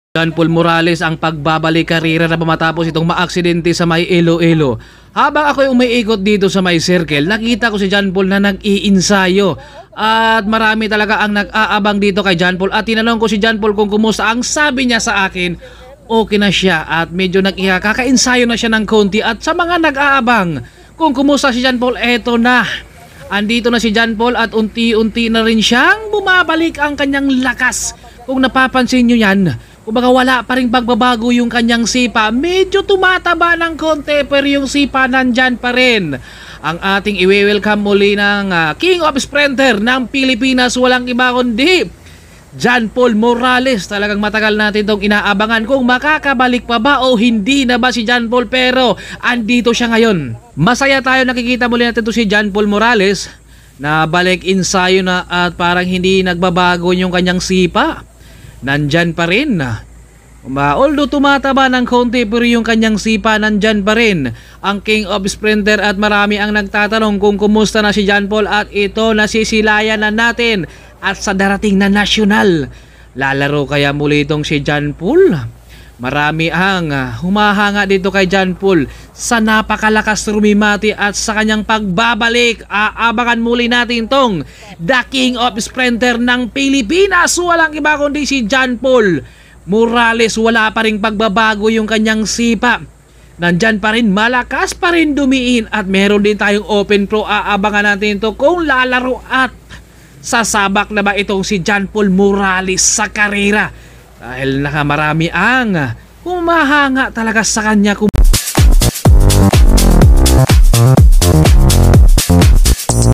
Jan Paul Morales ang Pagbabalik sa karera matapos ang trahedya sa Ilo-Ilo
Visit my Facebook Page at:
Whitemix Motovlog

Excellent Noodles Cycling Team
PRO TEAM